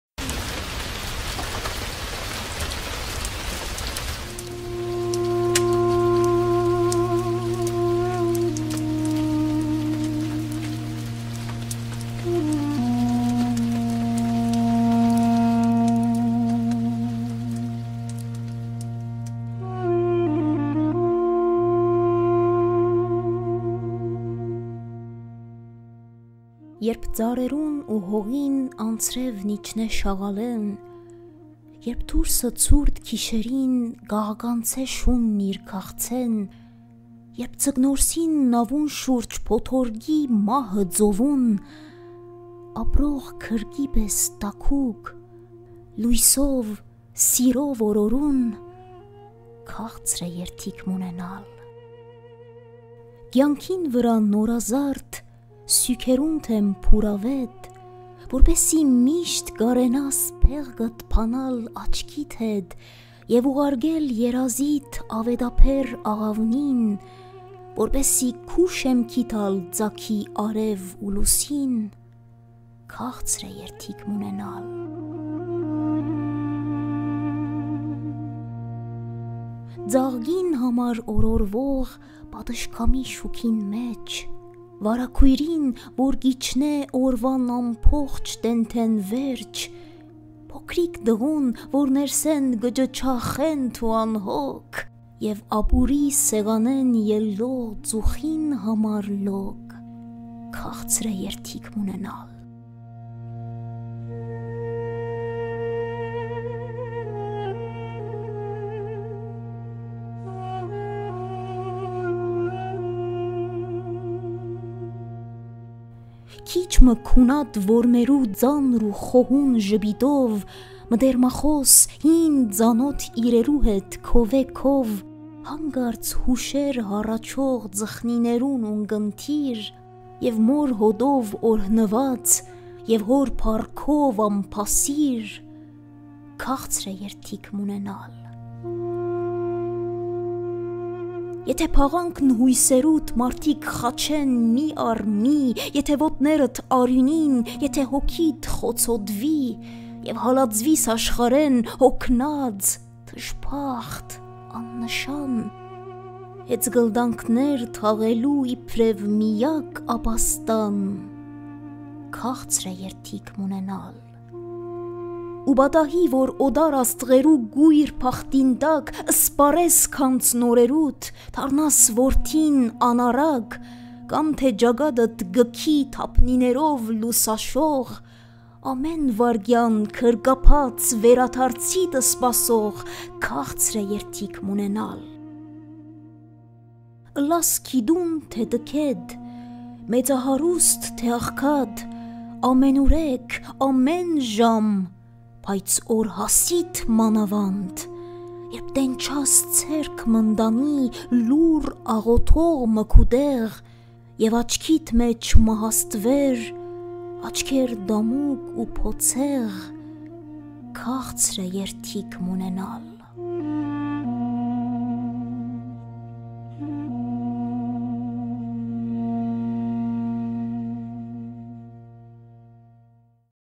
Քաղցր Է Երդիք Մ՚ունենալ - Մուշեղ Իշխան
Թող ո՛չ մէկ մարդ, ո՛չ մէկ ընտանիք զրկուի իր երդիքէն...

Մուշեղ Իշխան այս բանաստեղծութեան մէջ կը նշէ գրեթէ բոլոր այն բաները, որոնց համար քաղցր է երդիք մ՚ունենալ: